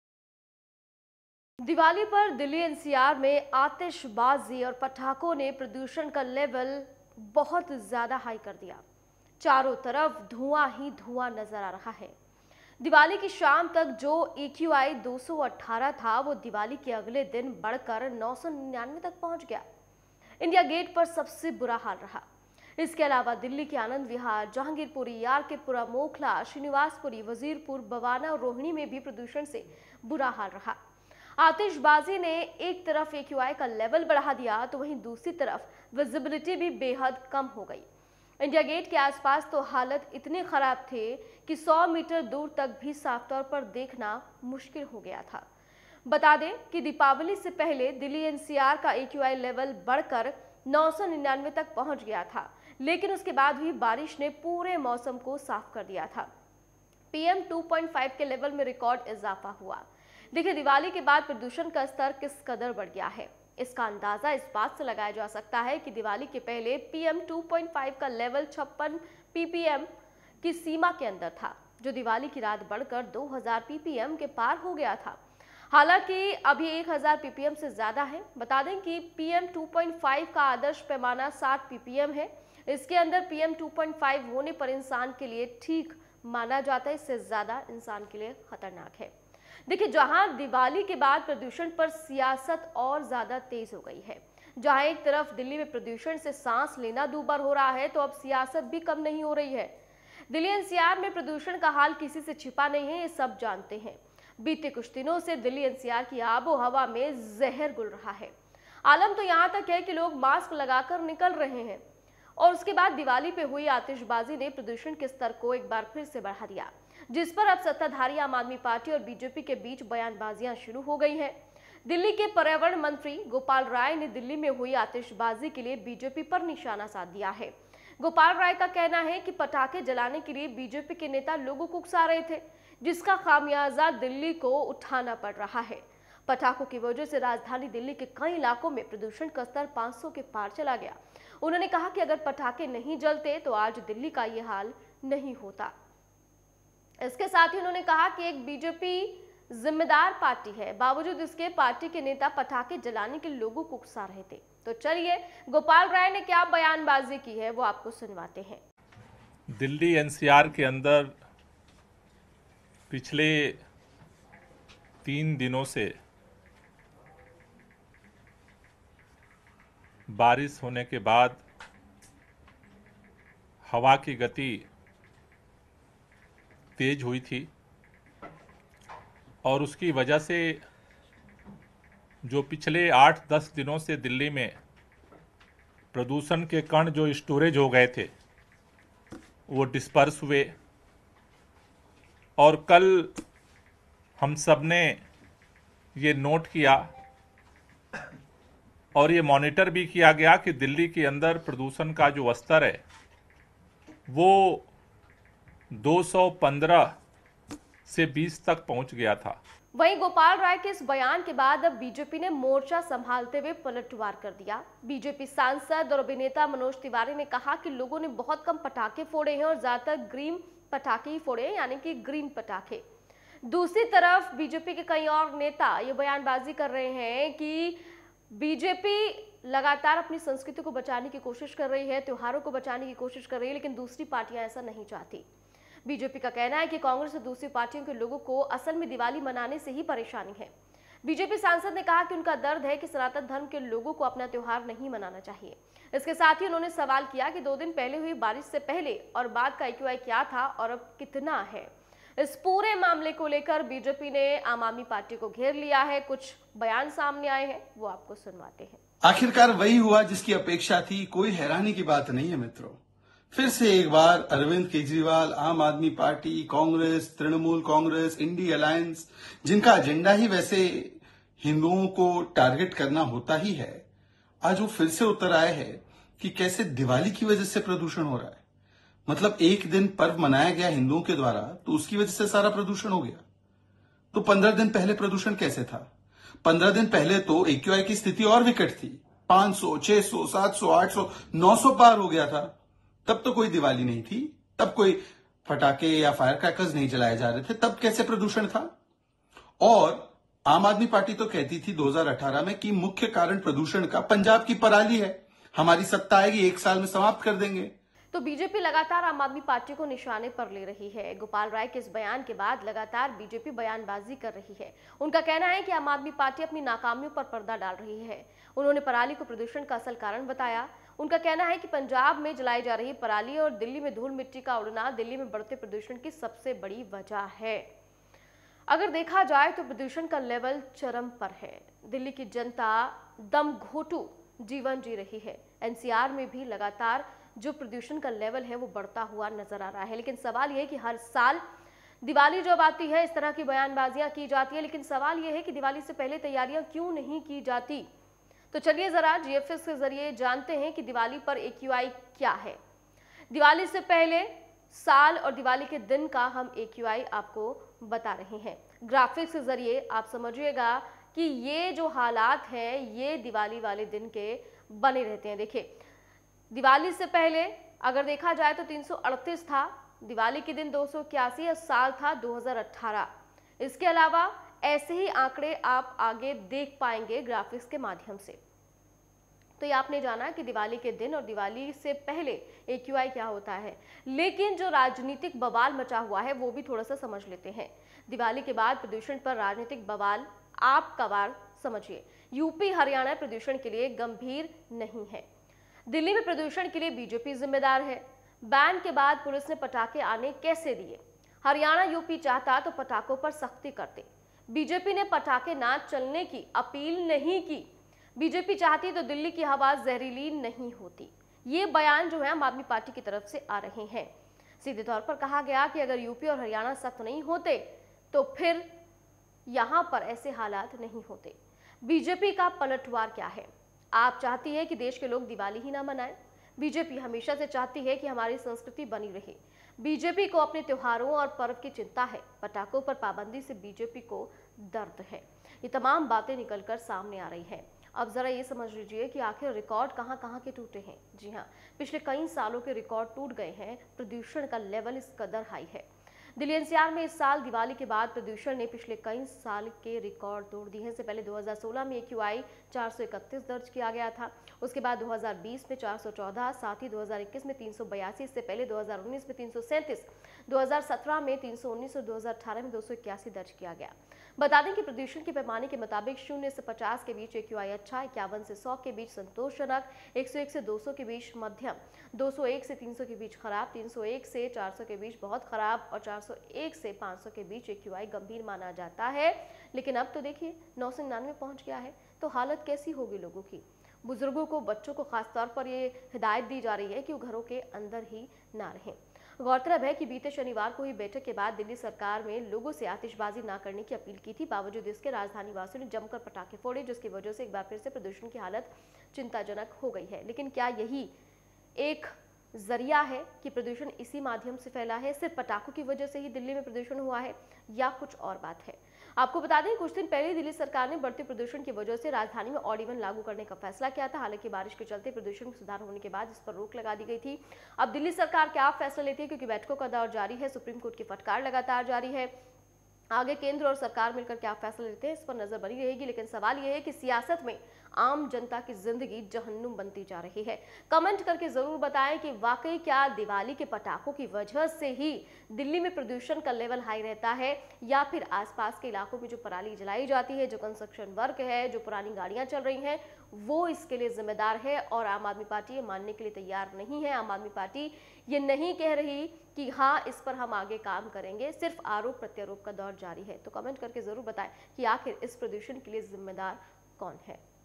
दिल्ली में प्रदूषण बीजेपी की वजह से हुआ- AAP
दिल्ली में हुई आतिशबाज़ी, क्या BJP ने कराई ? दिपावली पर बढ़ा प्रदूषण, क्या इसका है सियासी कनेक्शन ? दिल्ली की हवा के साथ-साथ सियासत में भी घुला ज़हर